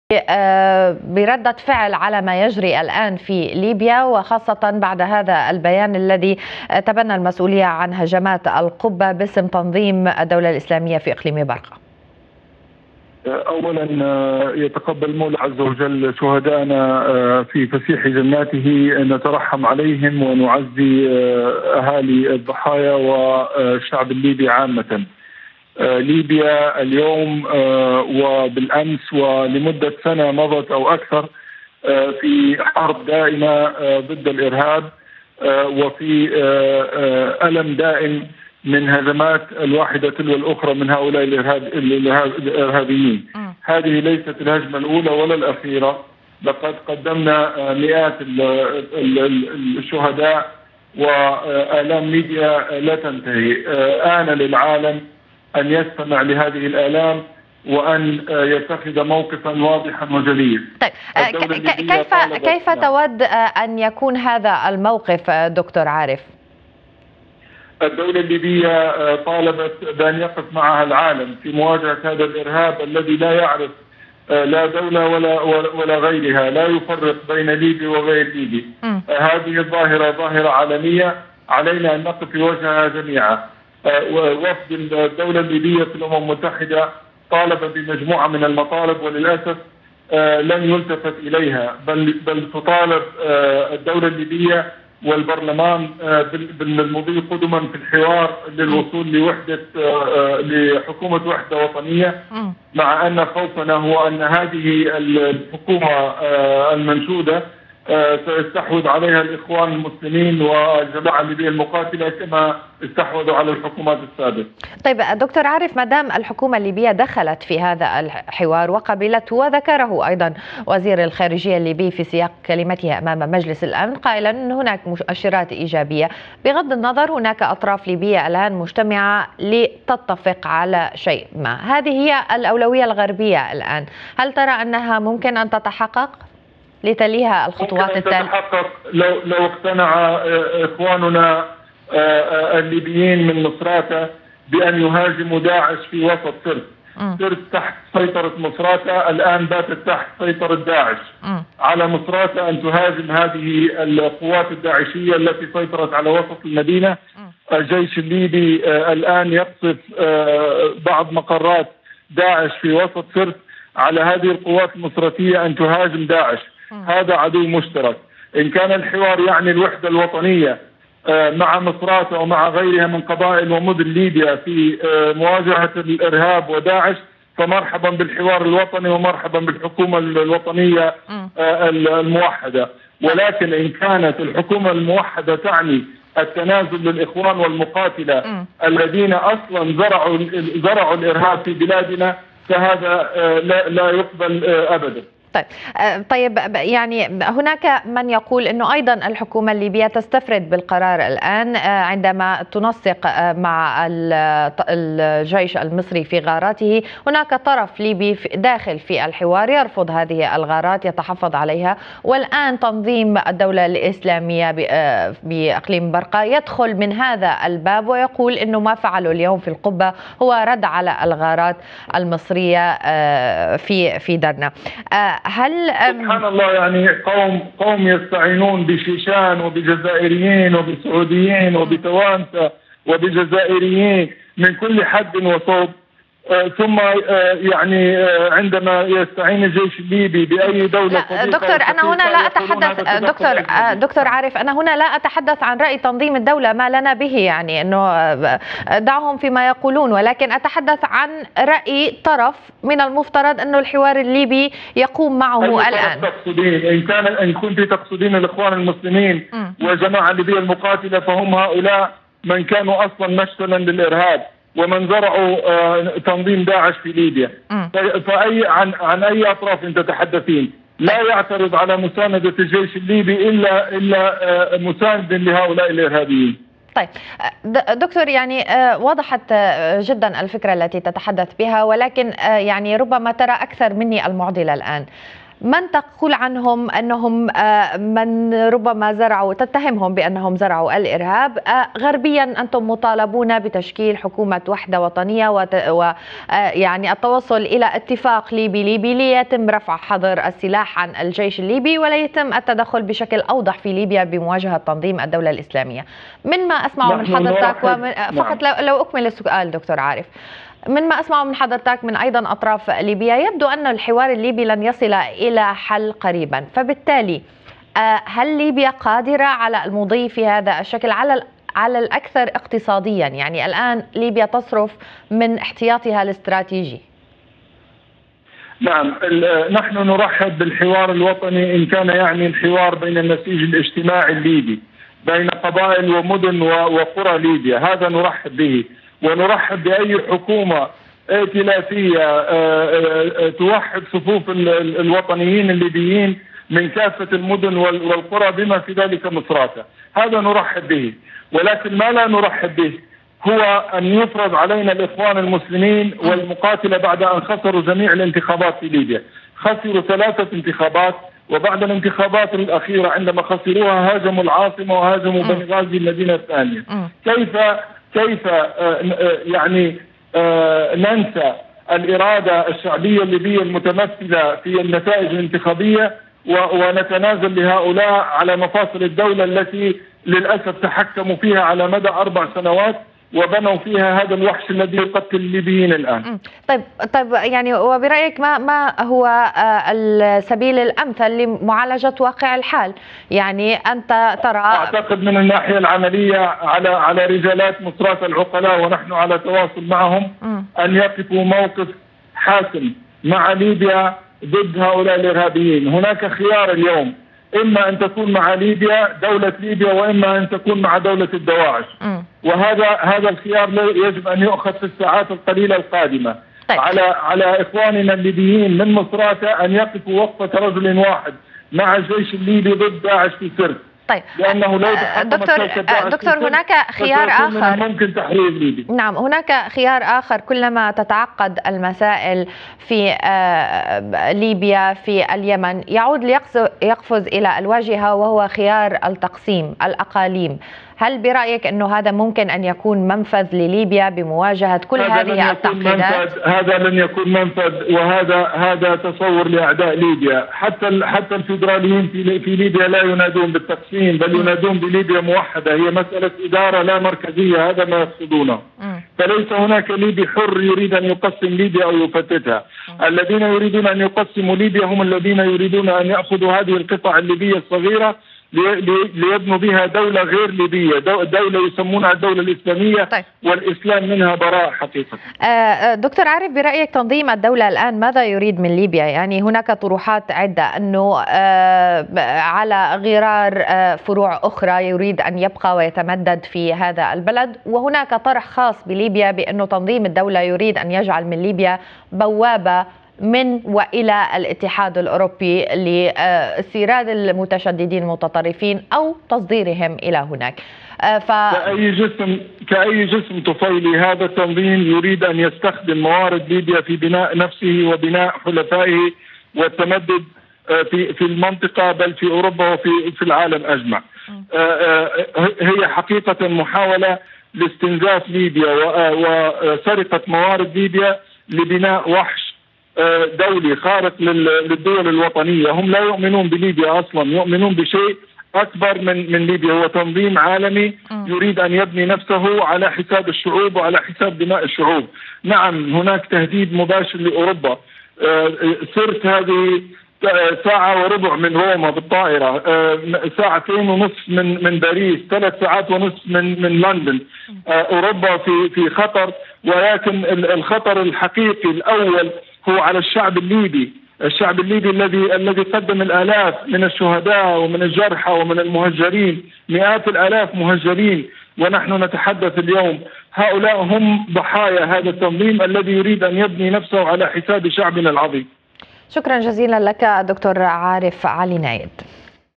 Dr. Aref Interviewed on BBC Arabic
Dr. Aref Ali Nayed interviewed about the current situation in Libya.